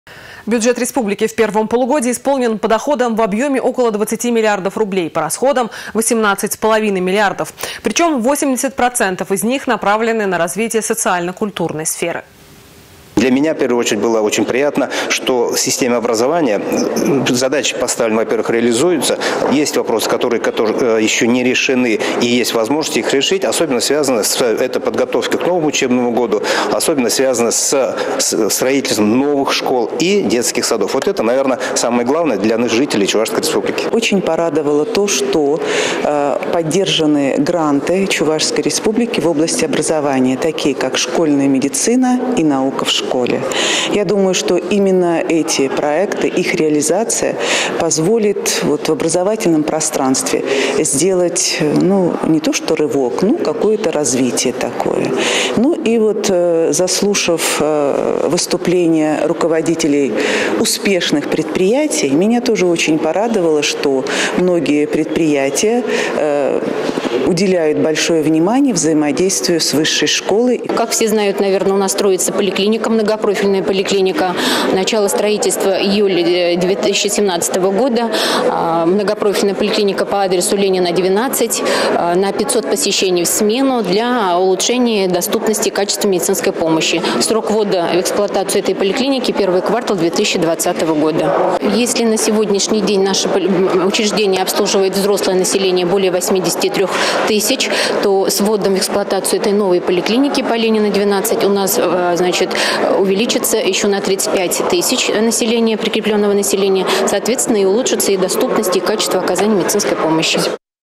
Бюджет республики в первом полугодии исполнен по доходам в объеме около 23 миллиардов рублей
По расходам — 18 с половиной миллиардов, причем 80% из них направлены на развитие социально-культурной сферы.